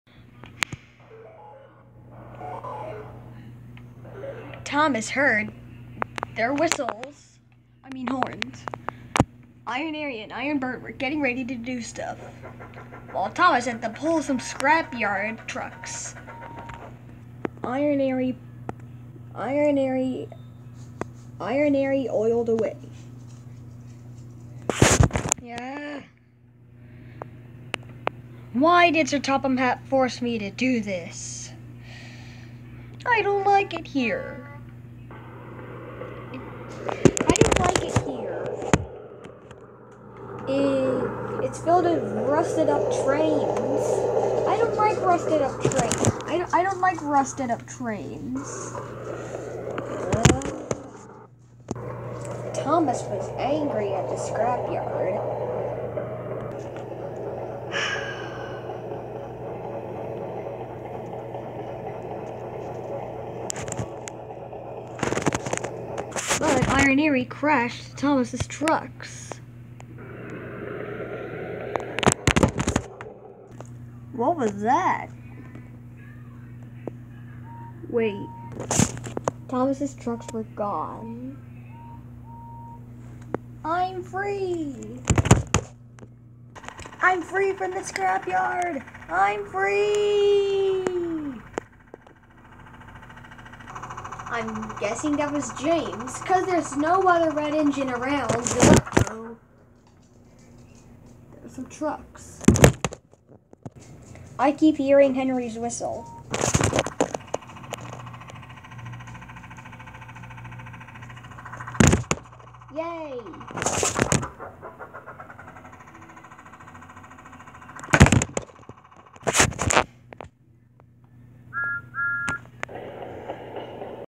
THOMAS AND FRIENDS CRASH SERIES EPISODE 60: IRON ARRY CRASHES THOMAS'S SCRAP TRUCKS
You might here Henry's whistle in the background